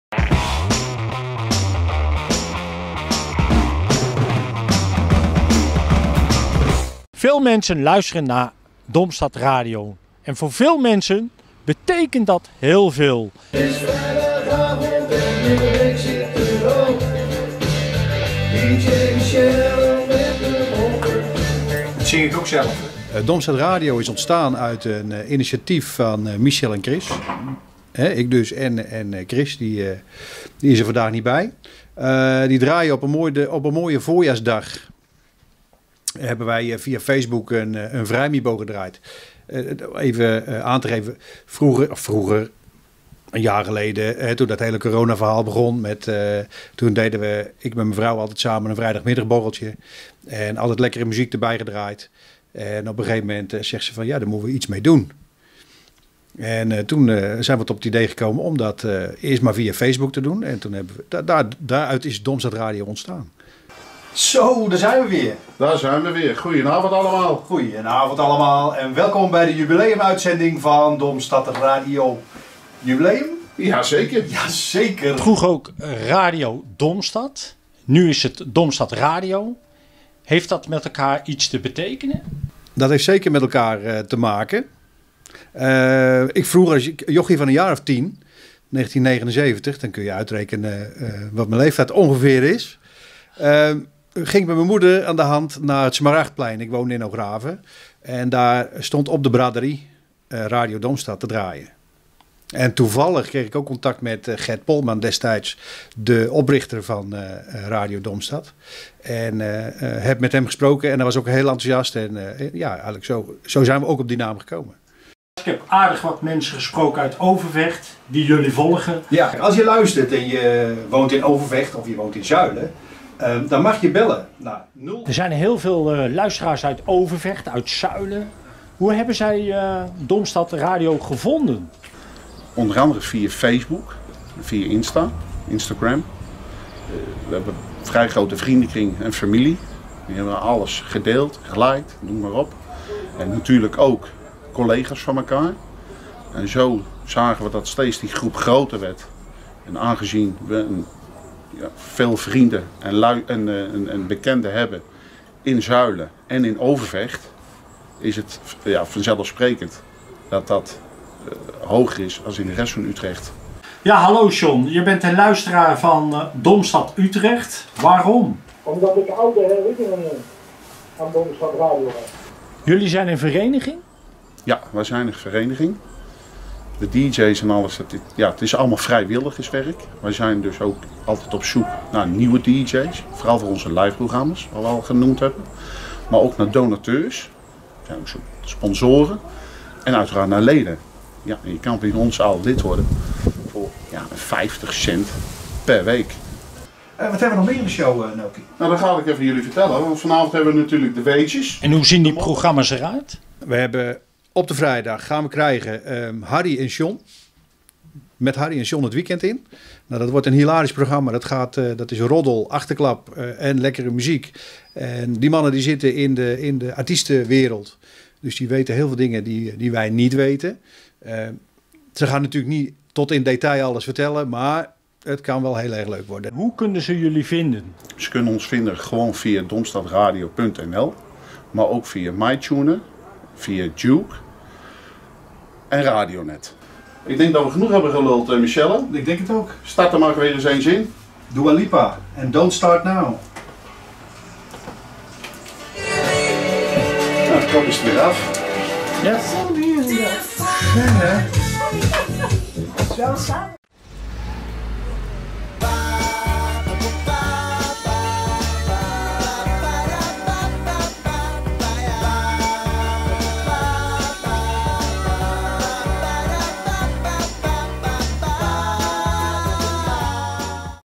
Domstad Radio
Verslaggever Leo Stroeven ging op bezoek bij de drijvende krachten achter Domstad Radio; populair in Overvecht en Zuilen en omstreken. Ontstaan uit berichten op Facebook, en tegenwoordig een vereniging met leden, vrijwilligers, donateurs en sponsoren. Ooit was er, in de jaren ’70 een Radio Domstad op het Smaragdplein in Hoograven; de ware inspiratiebron waaruit Domstad Radio van nu ontsprong. Vrijwilligers gezocht!